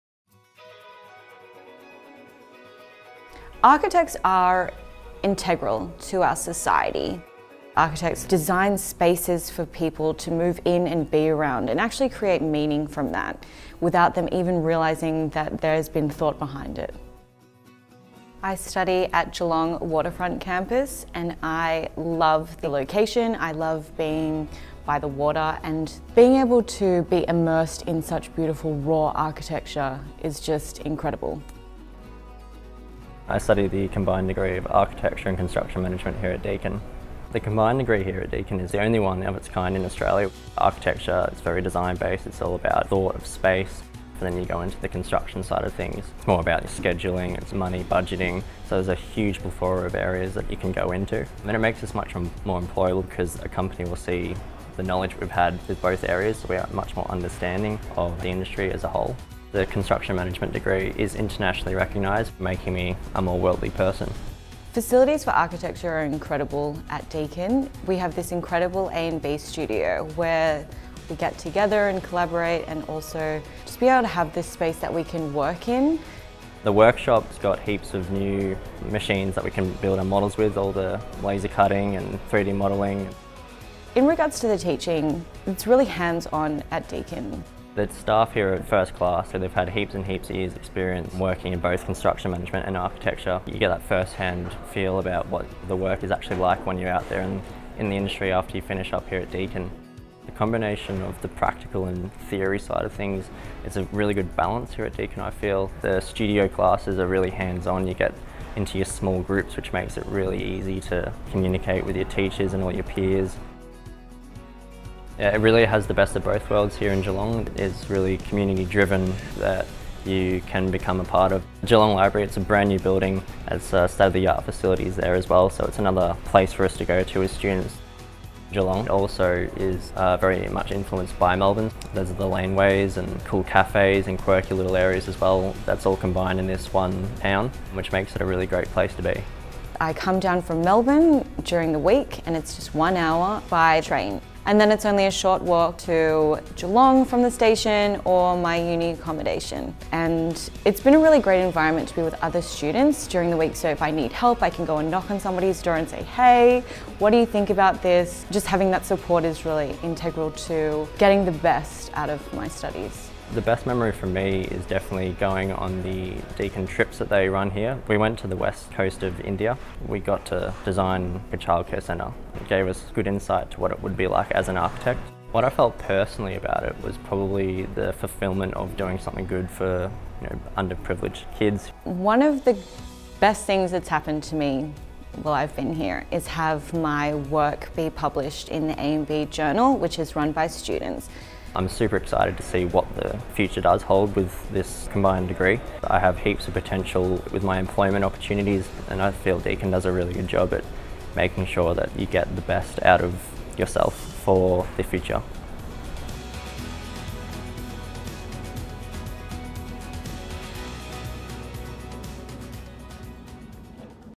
Architecture and Construction Management in Geelong
Study Architecture and Construction Management in Geelong's inspiring coastal location. Immerse yourself in a UNESCO city of design, with renowned lecturers, cutting-edge facilities and innovative architecture.